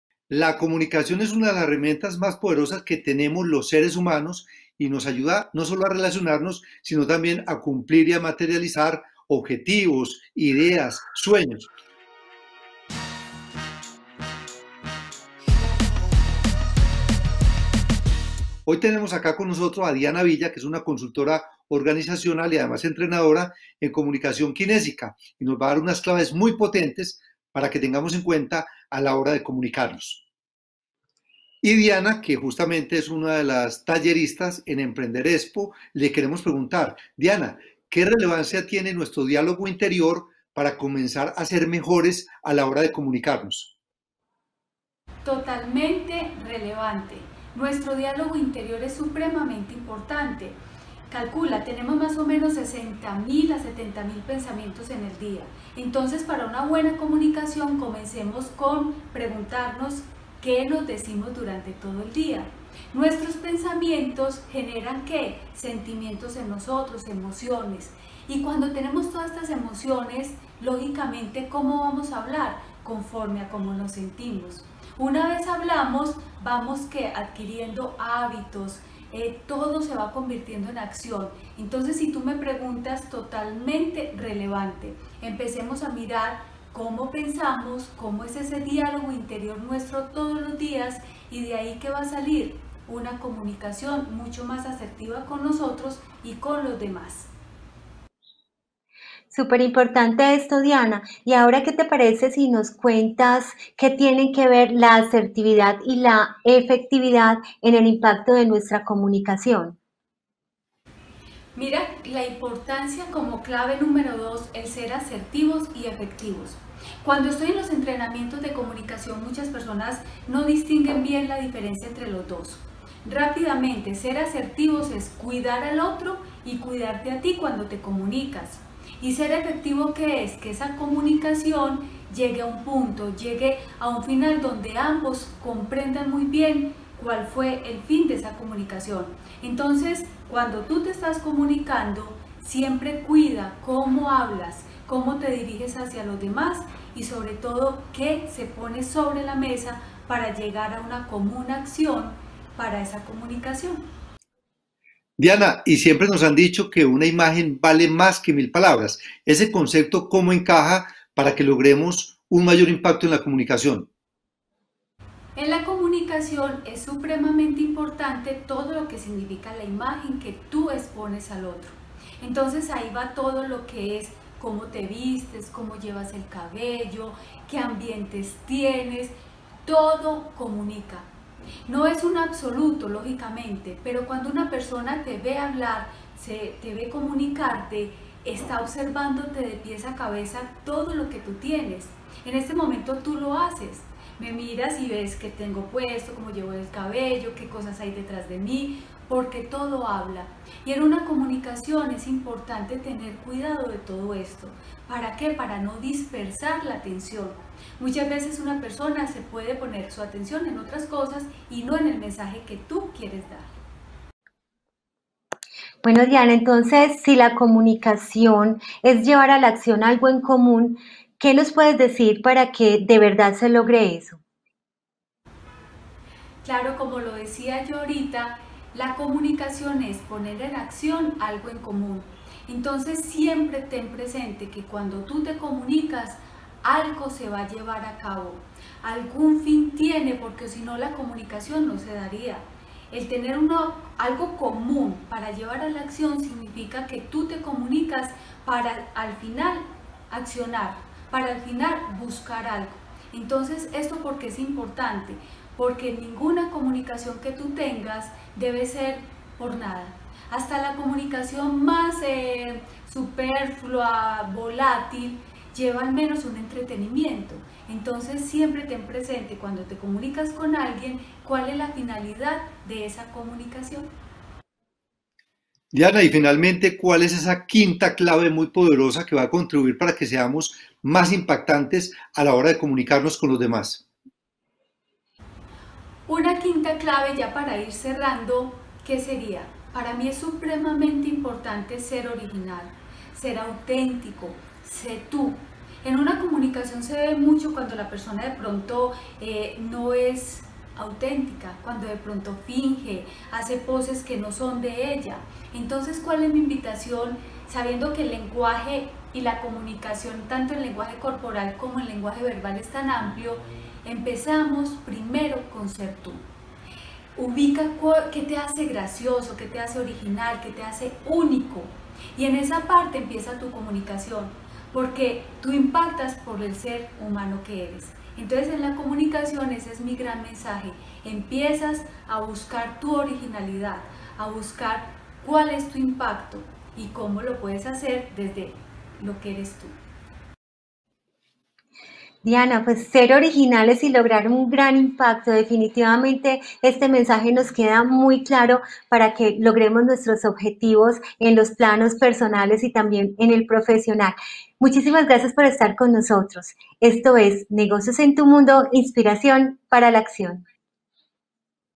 Claves para impactar al comunicar - [Negocios en Tm] ®
La comunicación es una de las herramientas principales de los seres humanos y es un medio desde el comienzo de nuestra historia para lograr, no solo relacionarnos, si no llegar a materializar ideas y objetivos. Diana Villa, Consultora Organizacional y Entrenadora en Comunicación Kinésica, nos dará unas claves para impactar a la hora de comunicarnos.

Negociosentm